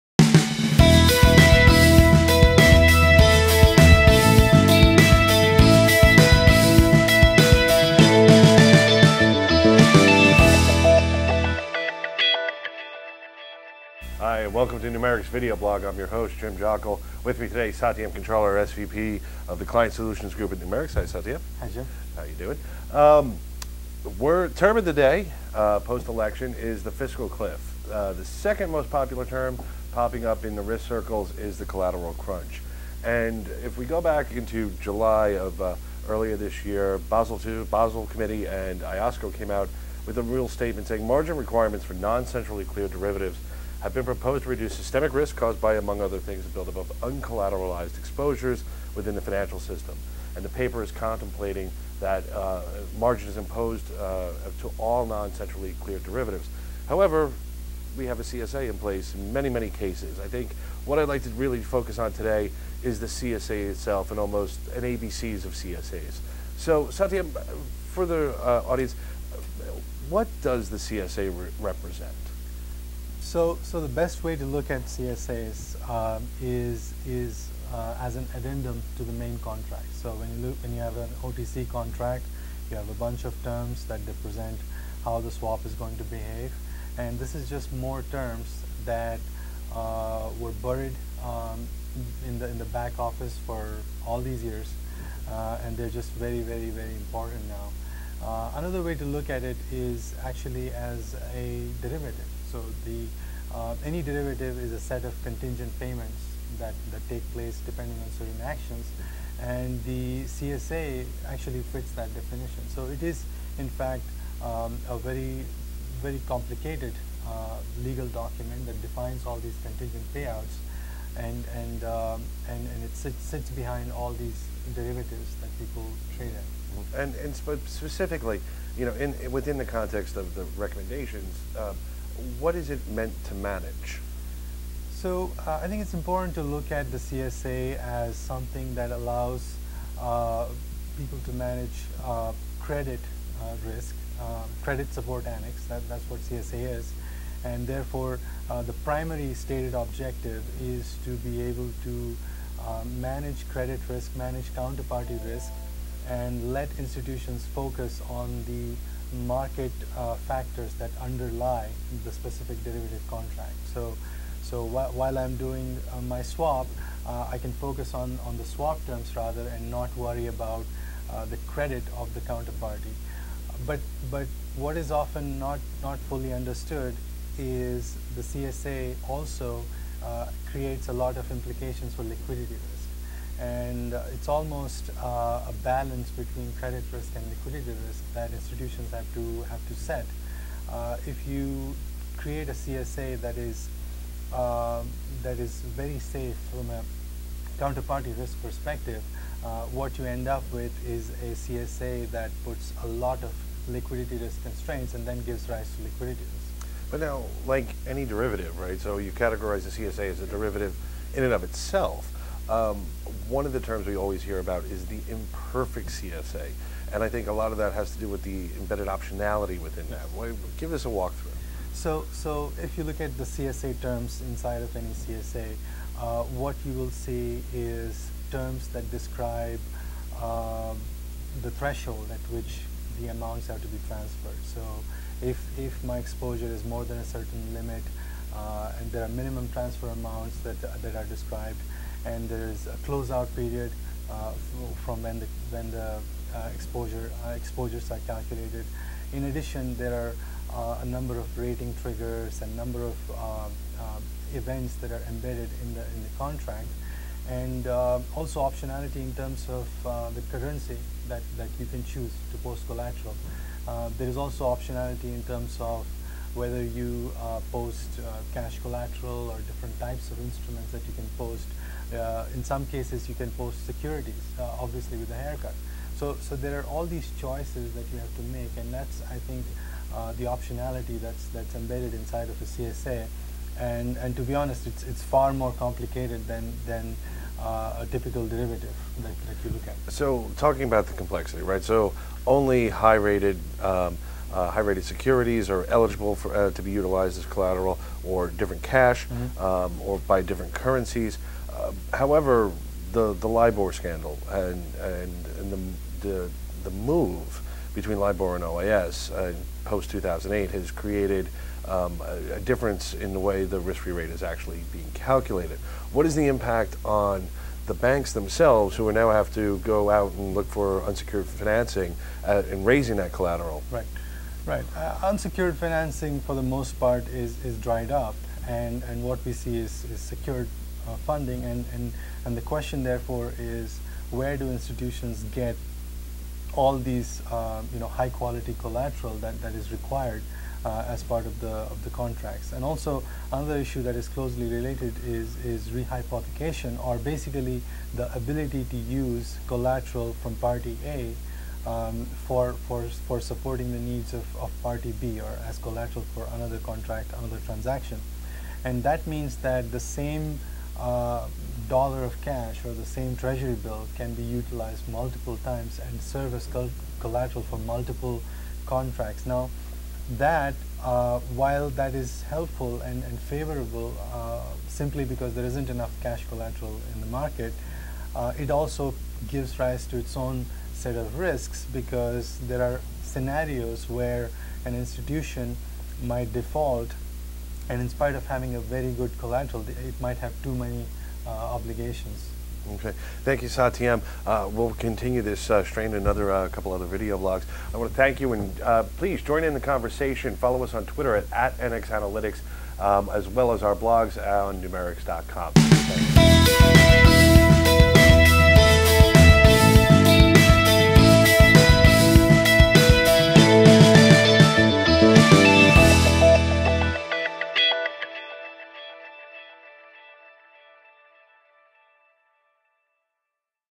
The ABCs of CSAs
In this video blog, Numerix Host James Jockle and Satyam Kancharla explore ISDA Credit Support Annex (CSA) agreements -- including what it represents and how it's used to manage Credit and Counterparty Risk.  Jim and Satyam also discuss the complexities of CSA terms, the imbedded optionality and the implications for liquidity risk.